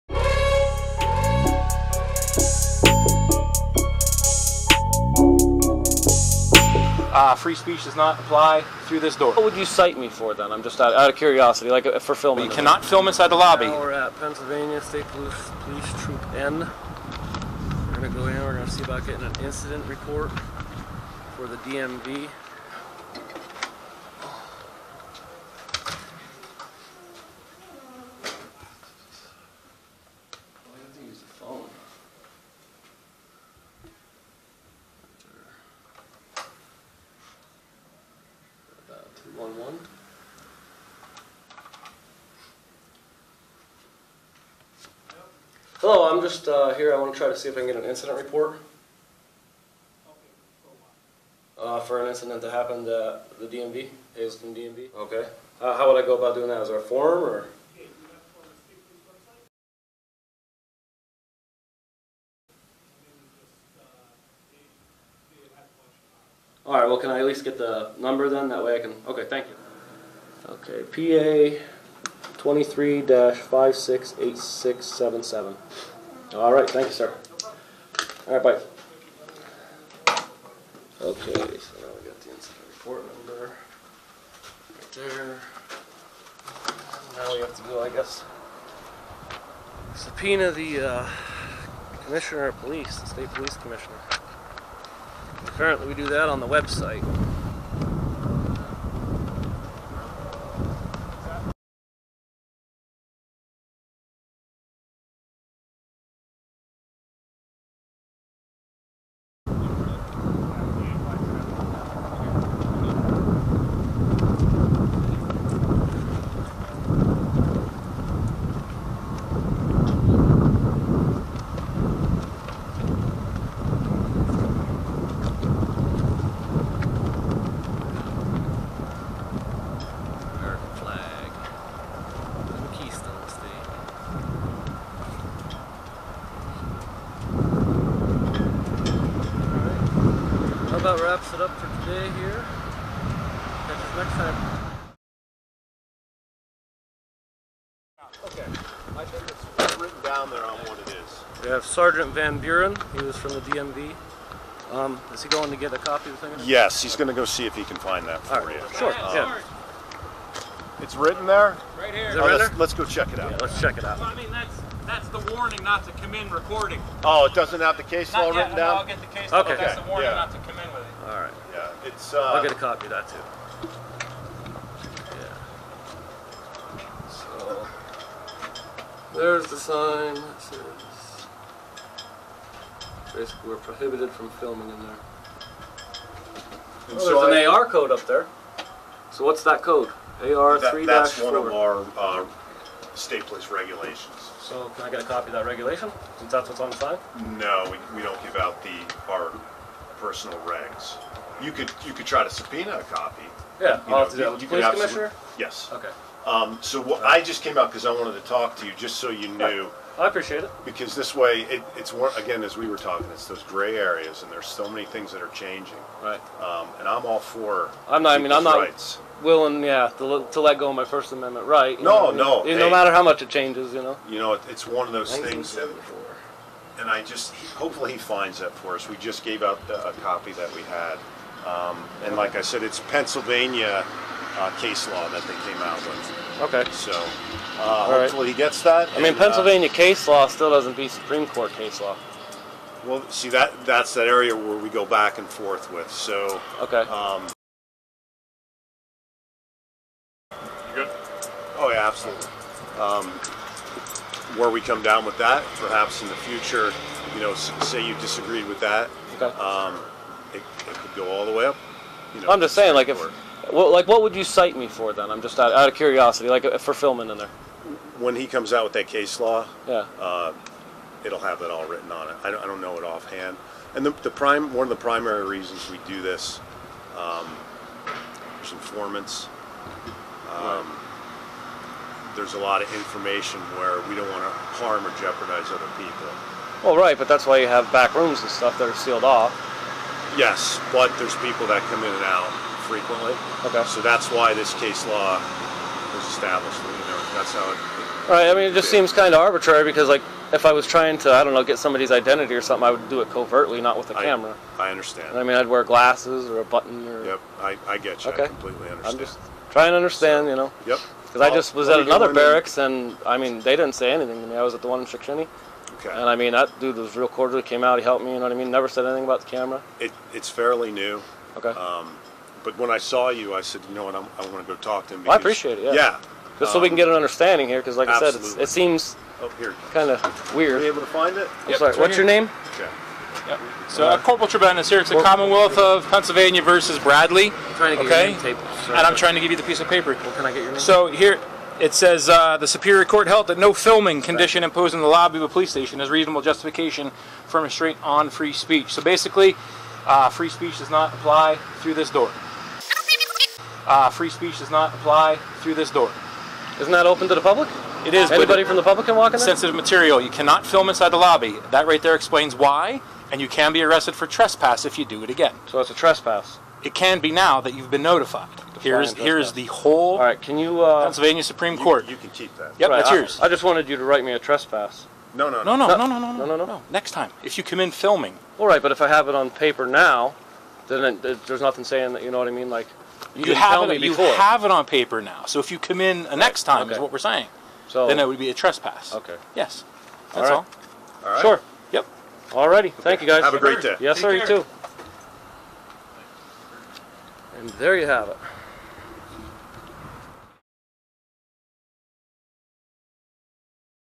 You DON'T HAVE RIGHTS in there!!!!
Hazleton State Police
Threatened Trespass
Case Law!!!

video from King of the Ruckus...check him out!